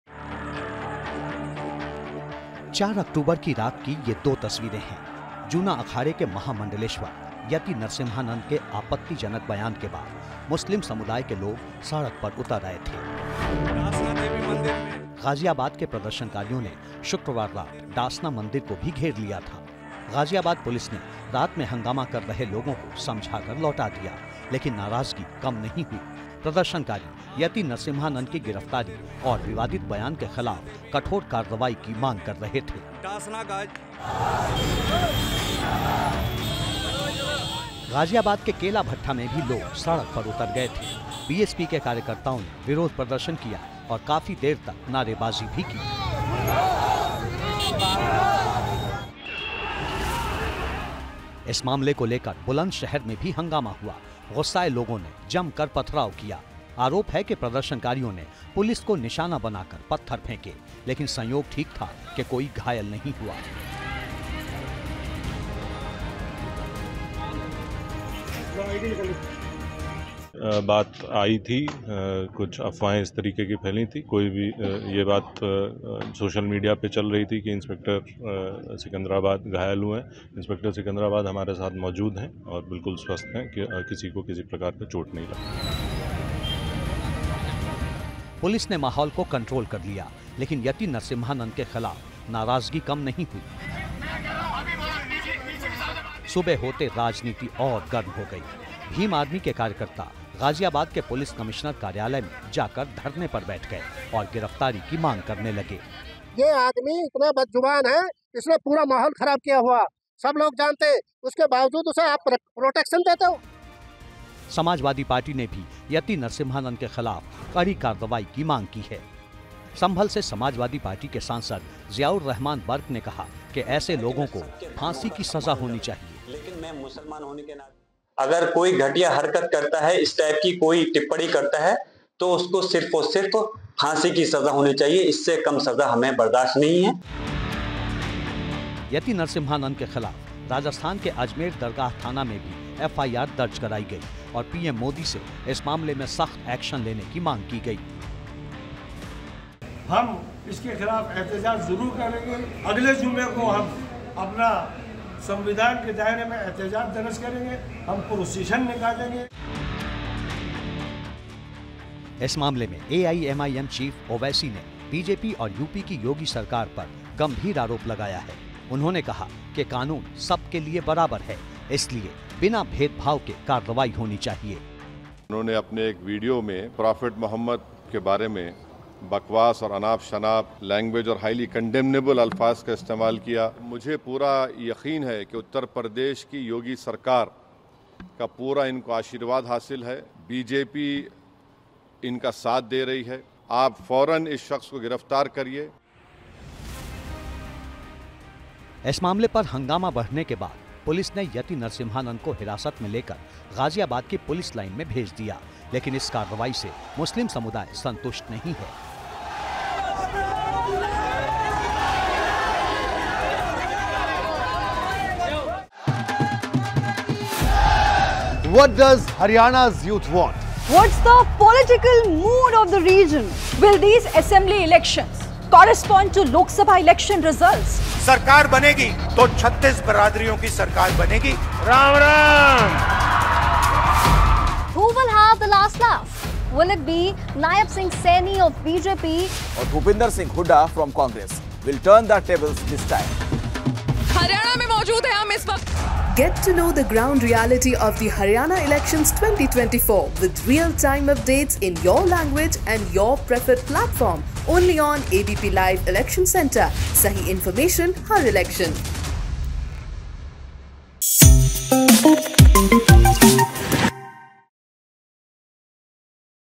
Yati Narsinghanand के बयान पर शहर-शहर संग्राम.. यूपी टू राजस्थान... कठोर एक्शन की डिमांड! ABP Live
ग़ाज़ियाबाद के डासना शिवशक्ति धाम के महंत और जूना अखाड़े के महामंडलेश्वर यति नरसिंहानंद के बयान के विरोध में कई शहरों में प्रदर्शन किया गया...यूपी के बुलंदशहर और गाजियाबाद में लोग सड़क पर उतर कर नारेबाजी की.. बुलंदशहर में नाराज लोगों ने पथराव भी किया. हंगामा बढ़ने पर पुलिस ने यति नरसिंहानंद को हिरासत में ले लिया...अब इस मामले में सियासत भी गरमा गई है।